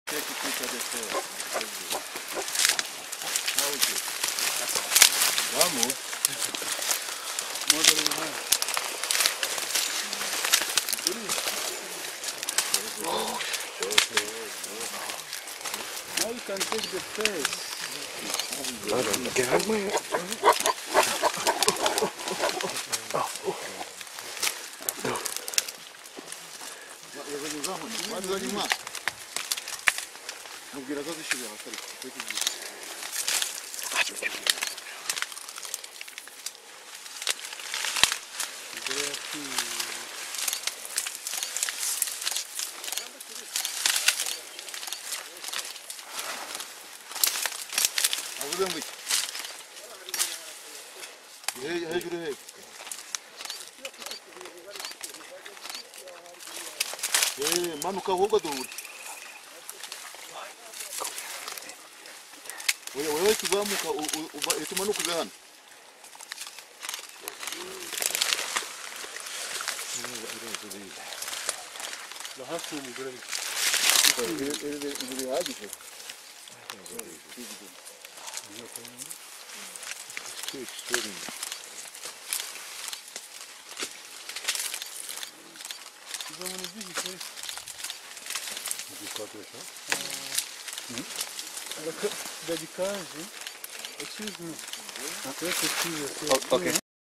Gorilla eating some plants
gorillas just hanging out and eating some veg